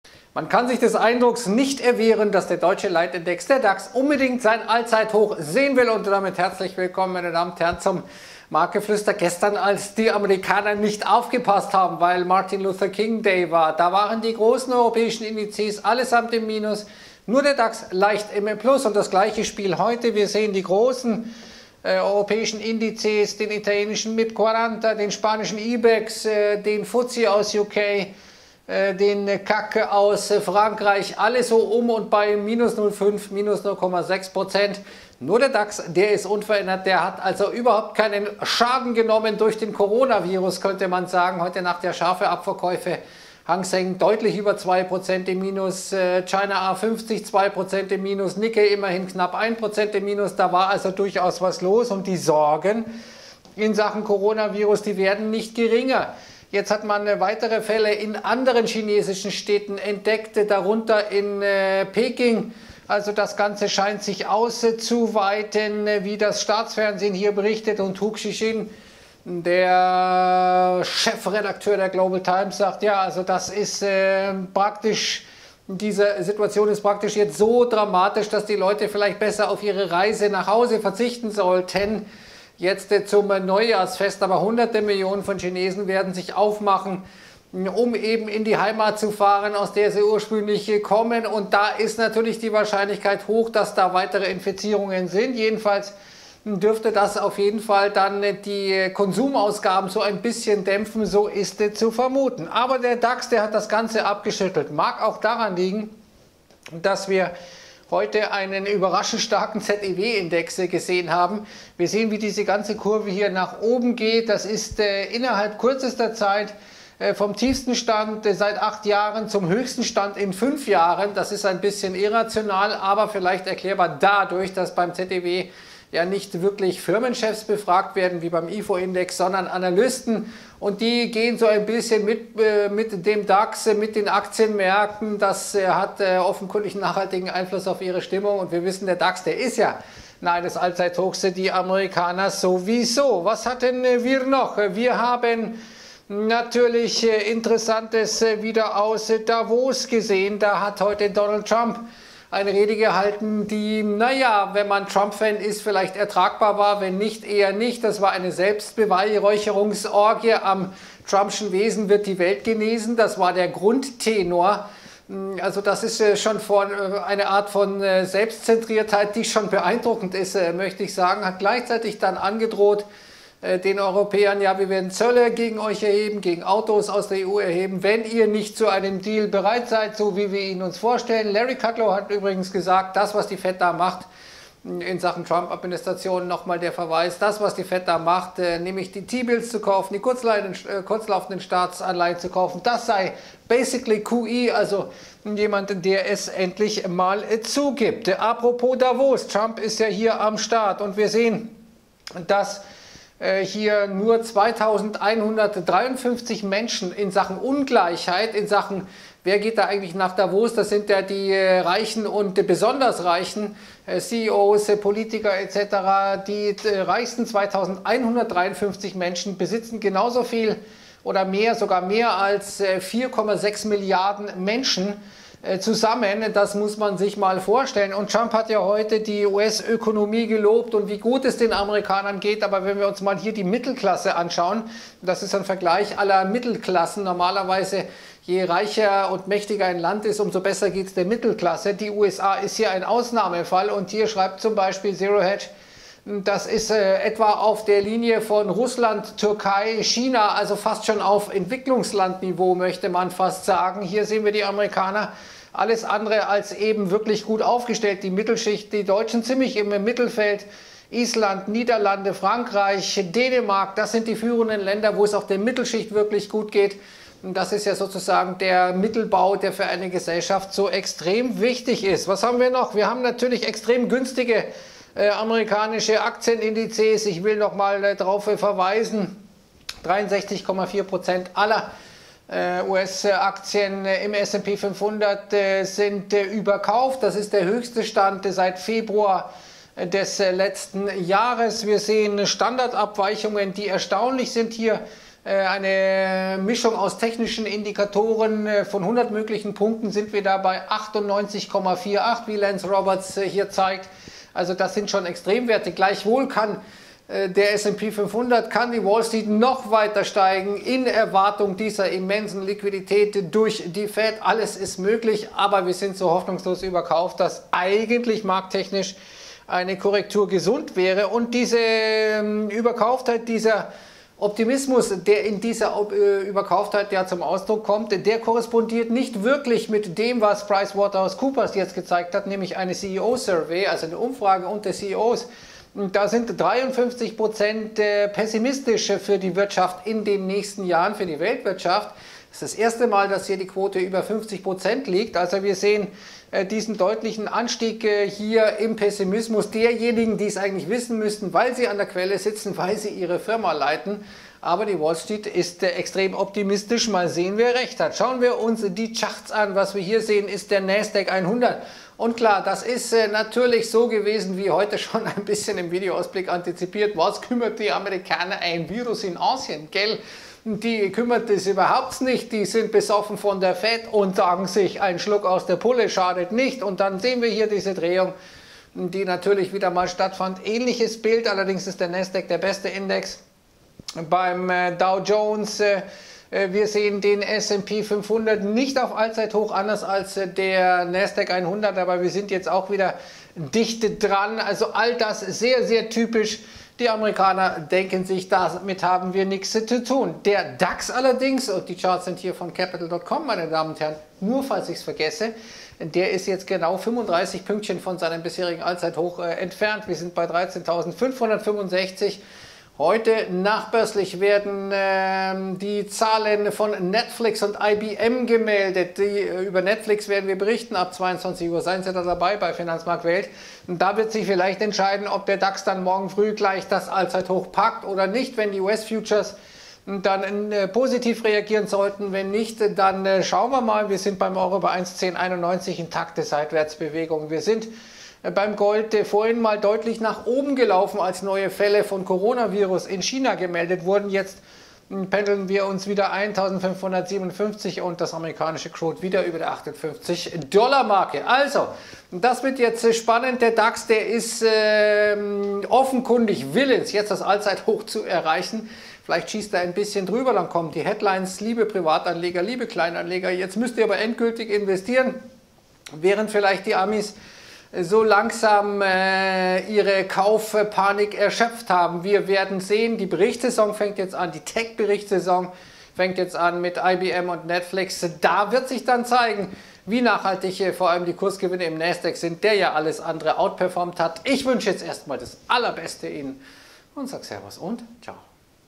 Dax will das Allzeithoch sehen! Marktgeflüster
Trotz der scharfen Verluste an den asiatischen Märkten schließt der Dax heute unverändert - und ist damit (wie schon gestern) mit Abstand der Beste der größeren europäischen Aktienindizes. Damit bleibt der Dax einen Wimpernschlag entfernt von seinem Allzeithoch - der Index will wohl dieses Allzeithoch unbedingt sehen, erkennbar an der relativen Stärke zu den anderen Indizes! Unterdessen häufen sich die Meldungen von weiteren Ansteckungen mit dem Coronavirus in China. Und Donald Trump hält eine exzessive Selbstbeweihräucherungs-Rede in Davos mit dem Tenor: am trumpschen Präsidentenwesen wird die Welt genesen. Gleichzeitig droht Trump der EU mit Autozöllen. Und die Wall Street ignoriert - wie hätte es anders kommen können - den Coronavirus..